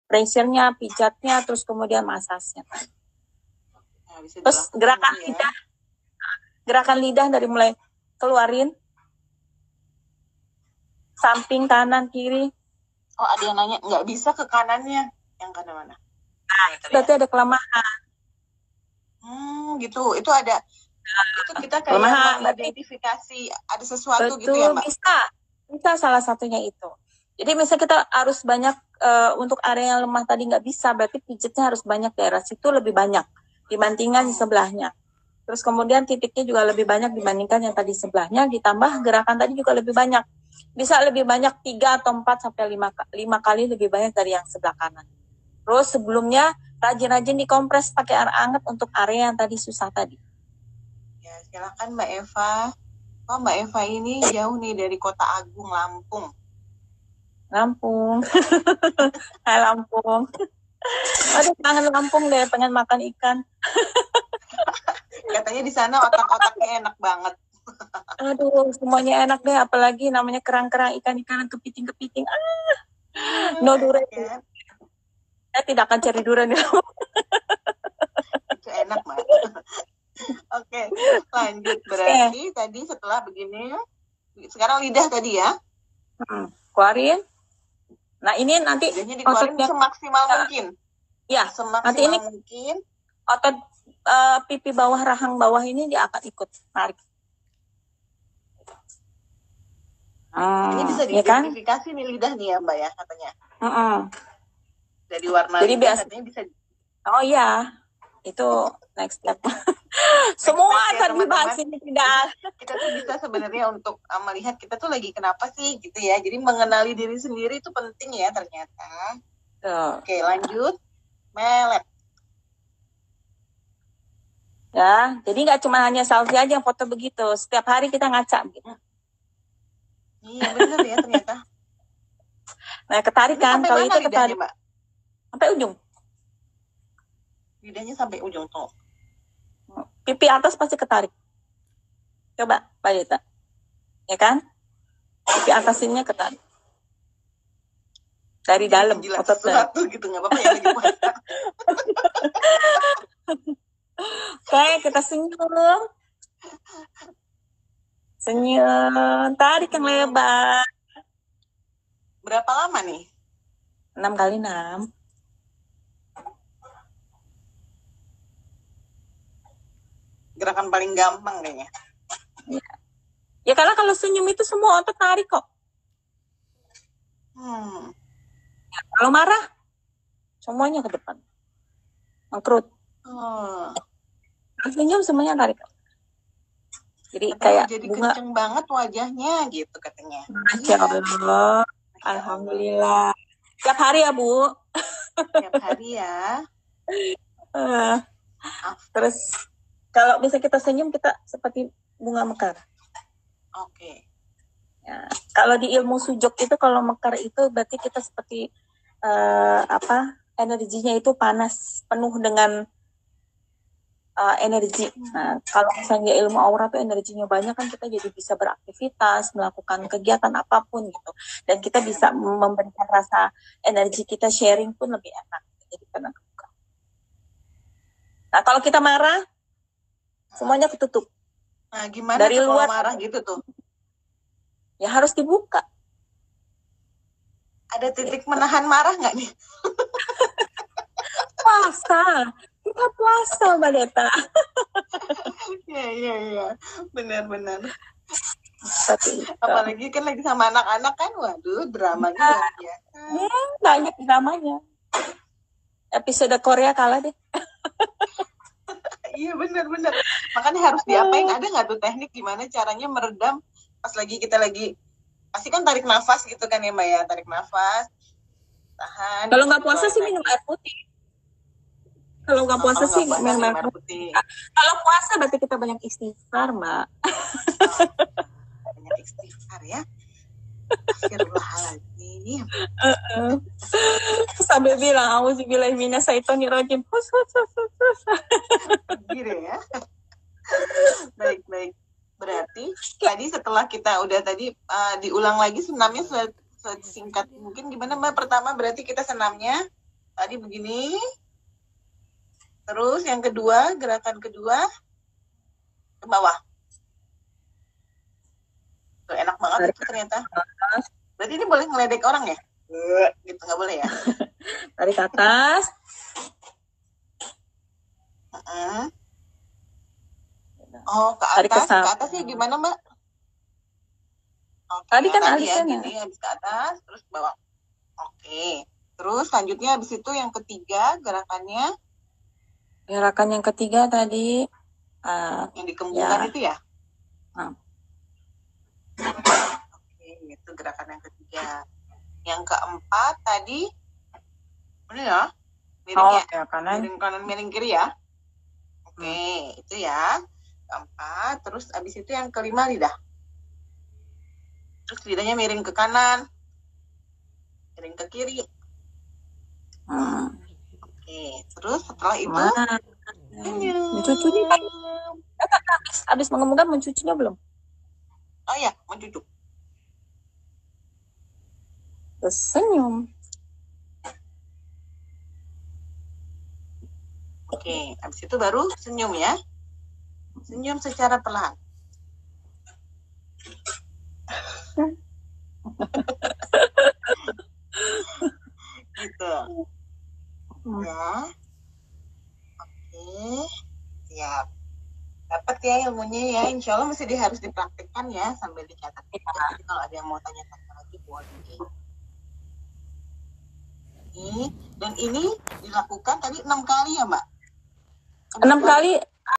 totok wajah untuk diri sendiri
dengan kita dapat mengenali titik pad wajah kita dapat melakukan untuk diri kita sendiri dan juga untuk orang orang yang ada cintai